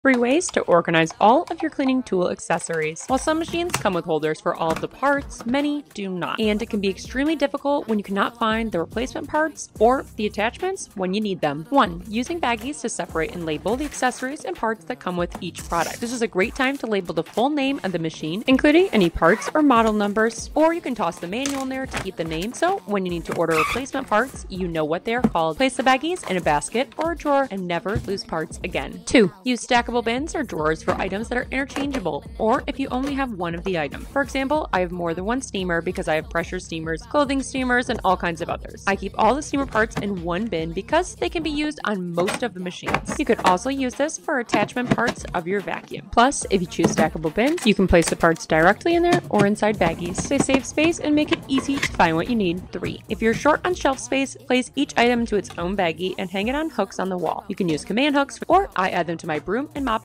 How to organize cleaning tools, vacuum parts, cleaning accessories and more!
✨3 ways to organize all your cleaning tool accessories and vacuum parts!✨

While some machines (vacuums, steamers, air purifiers, iron, etc.) come with holders for all of the parts, many do not! 😞And it can be extremely difficult when you cannot find the replacement parts or the attachments when you need them! 🙄

1)    Use baggies to separate and label the accessories and parts that come with each product.👛 This is a great time to label the machine fully with its model #, and any parts that are in there, so when you need to order replacements, you know what they are called. Or you can toss the manual in there to keep the name and any special information. Place the baggies in a basket or drawer and easily find them when you need them, never loose parts again!
2)    Use stackable bins or drawers for items that are interchangeable or if you only have one of them.😍 For example, I have more than one steamer because I have pressure steamers, clothing steamers, etc. I keep all the steamer parts in one bin because they can be used on almost all the machines. Or use stackable bins to do the same and save space! ‼️
3)    If you are short on shelf space, place each item into its own baggie and hang on hooks on the wall. 🪝You can use command hooks for the items you need, or I add them to my broom and mop organizer that has hooks for extra items! 🧹

Follow for more cleaning and organizing tips. ✨

Amazon Storefront